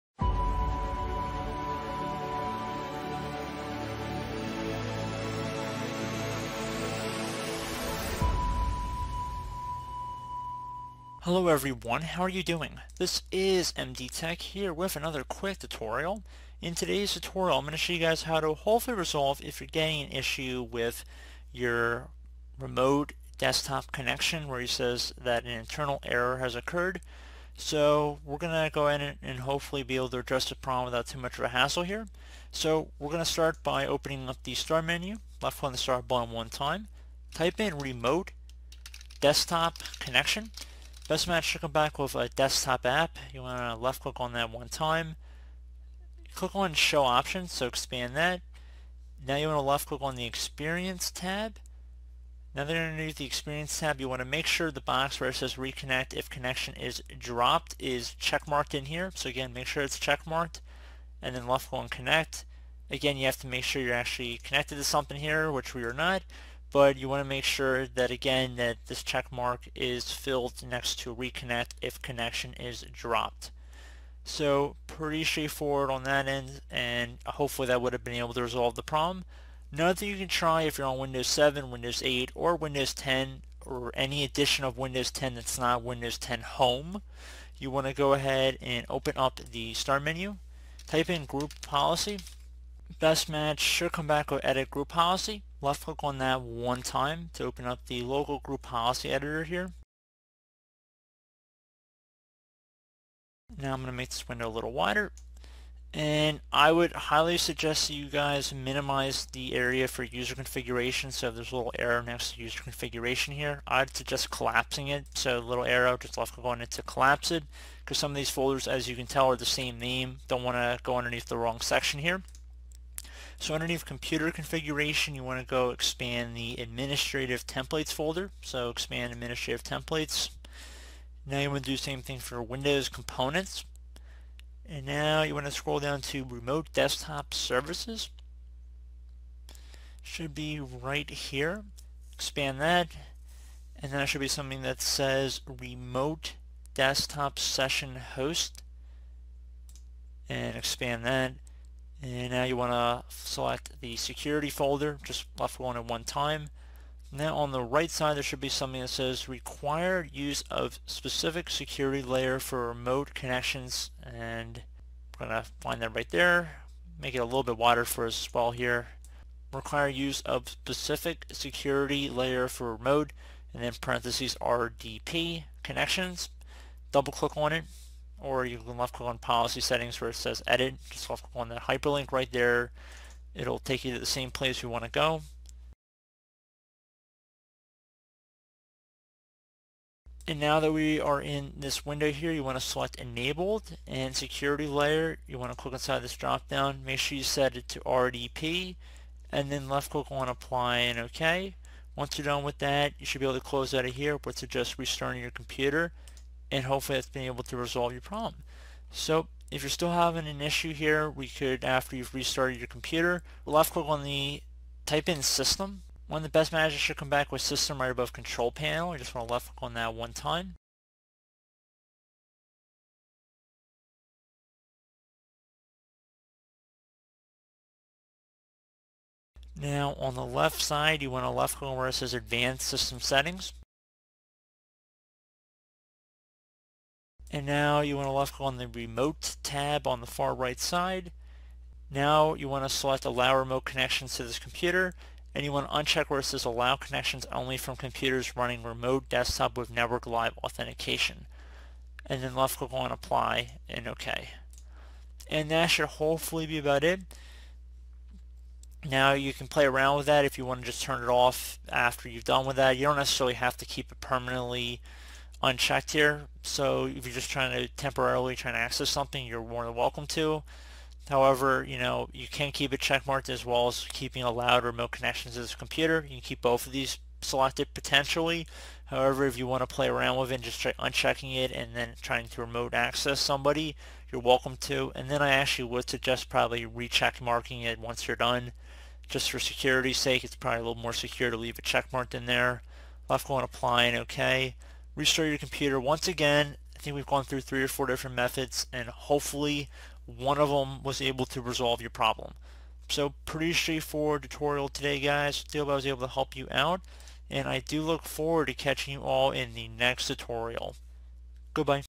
Podłączenie pulpitu zdalnego Wystąpił błąd wewnętrzny
Jak naprawić połączenie pulpitu zdalnego „Wystąpił błąd wewnętrzny” w systemie Windows 11/10/8/7 [samouczek]

Błąd pulpitu zdalnego „Wystąpił błąd wewnętrzny” jest często spowodowany przez ustawienia RDP lub zabezpieczenia lokalnych zasad grupy. Pojawiło się sporo raportów, które stwierdzają, że użytkownicy nie mogą używać klienta Podłączania pulpitu zdalnego do łączenia się z innym systemem. Według doniesień ten problem pojawił się nieoczekiwanie i nie był spowodowany żadną konkretną akcją.

Po kliknięciu Połącz klient usługi Podłączanie pulpitu zdalnego zawiesza się, a po kilku sekundach pojawia się błąd. Ponieważ Podłączanie pulpitu zdalnego jest używane przez wielu użytkowników do celów biznesowych lub osobistych, ten błąd może okazać się dość uciążliwy. Nie martw się jednak, ponieważ będziesz w stanie rozwiązać problem, przeglądając ten artykuł.